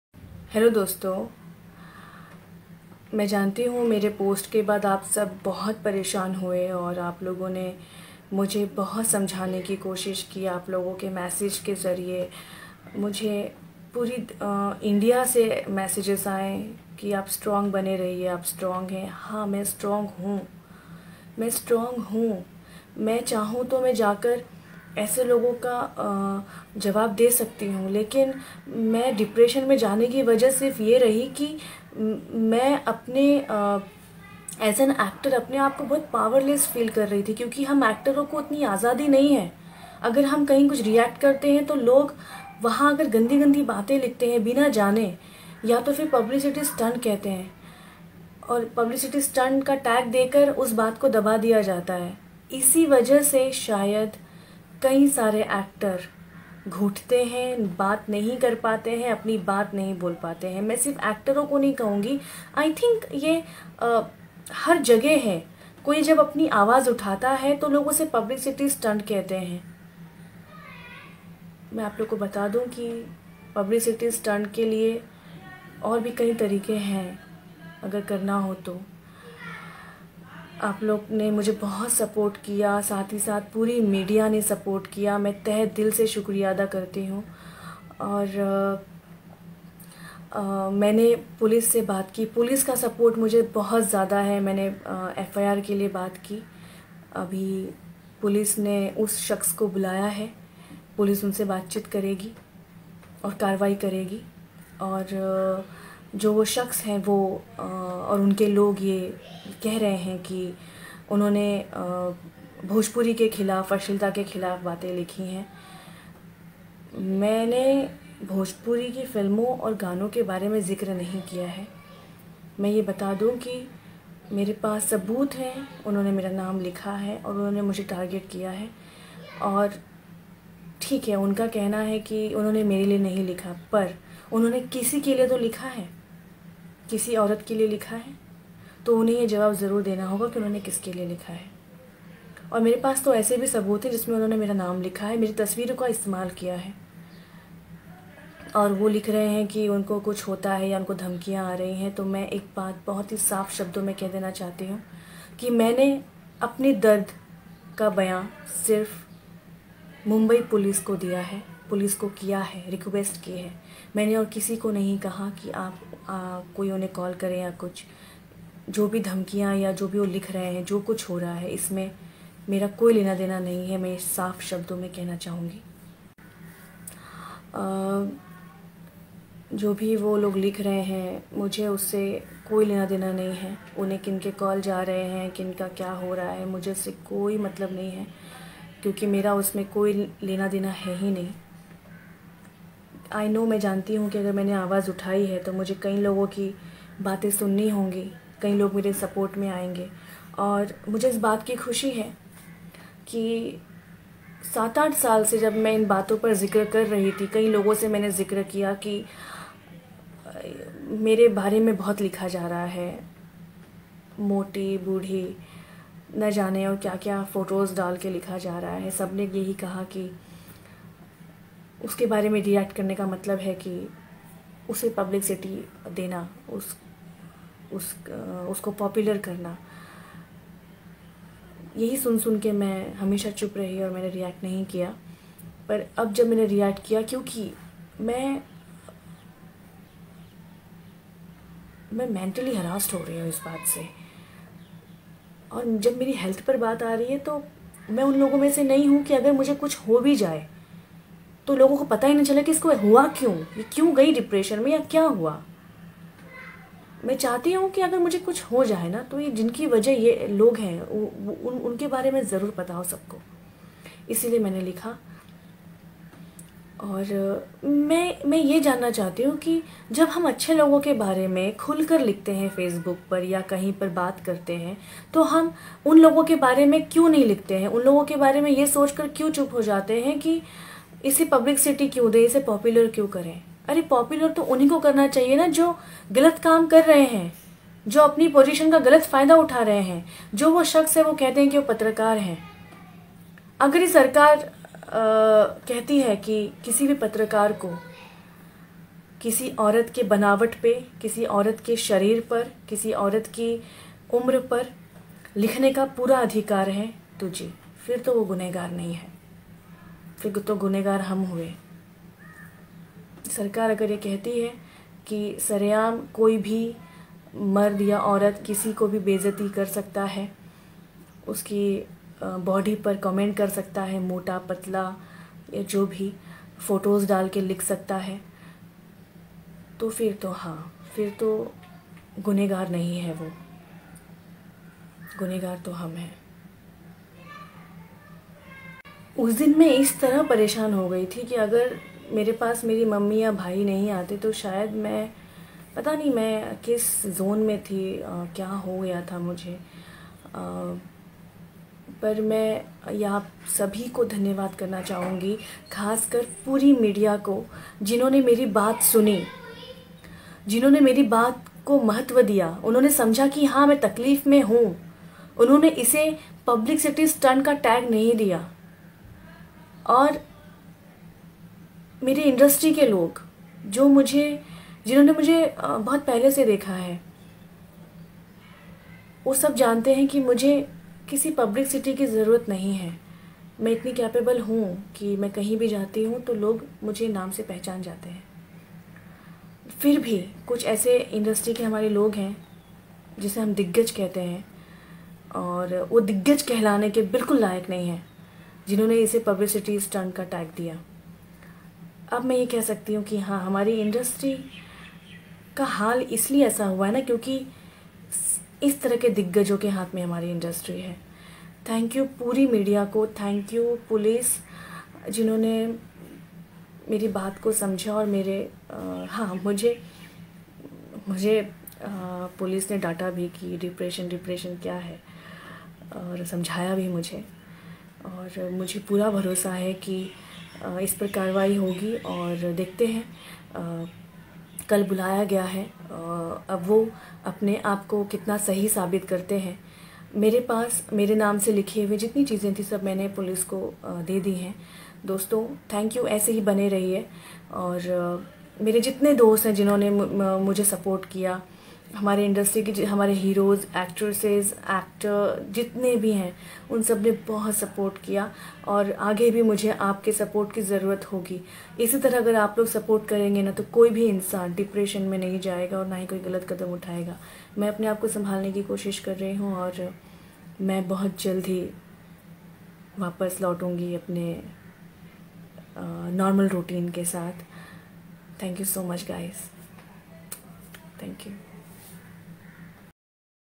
रानी चटरजी का नया वीडियो आया सामने, बोलीं एक्टर होने के नाते चुप रहना पड़ता है | Bindaas Bhojpuriya
रानी चटरजी का नया वीडियो आया सामने, बोलीं एक्टर होने के नाते चुप रहना पड़ता है Video